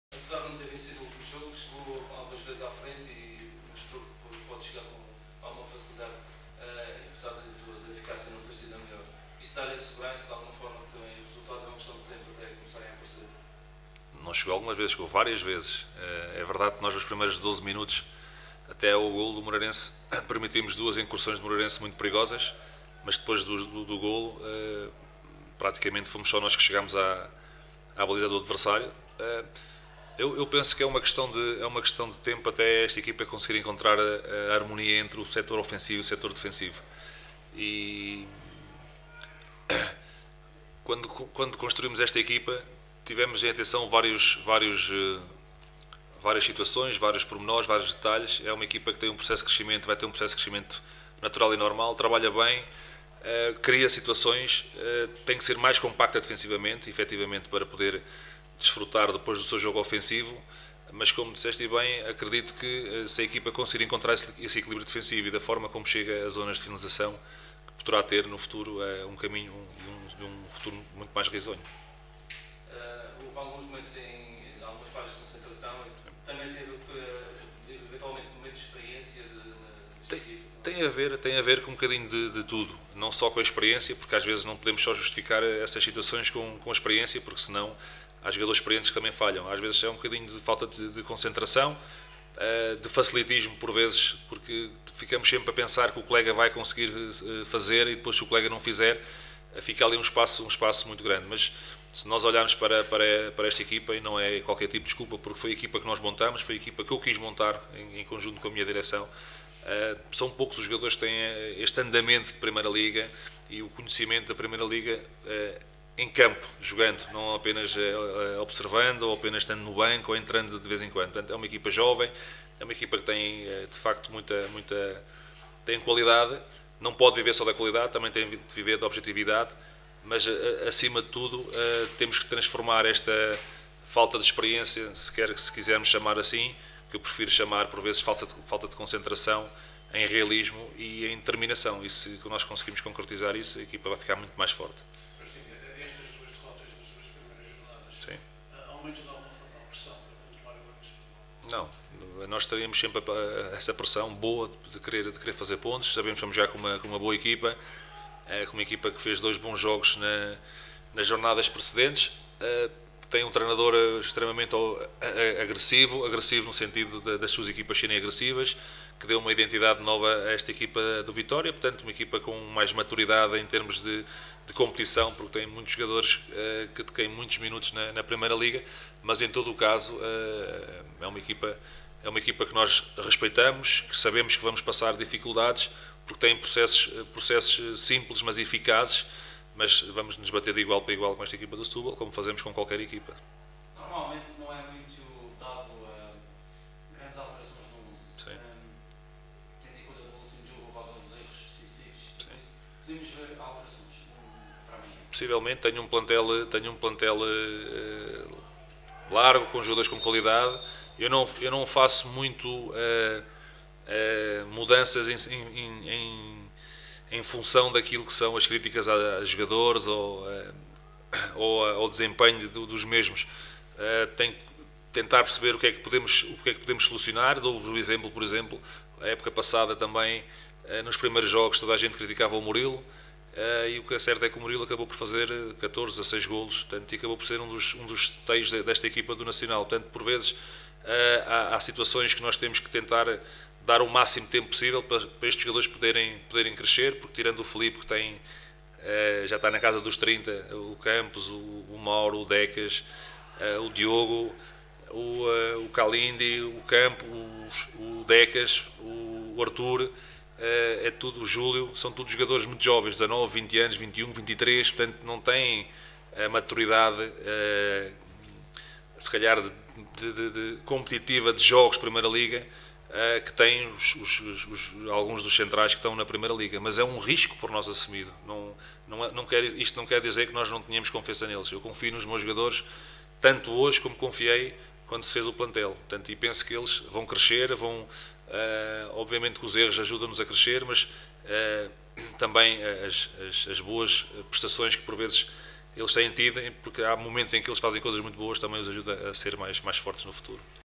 Costinha antevê Vitória Setúbal vs Nacional
Sala de Imprensa do Estádio da Madeira, 25 de Agosto de 2018.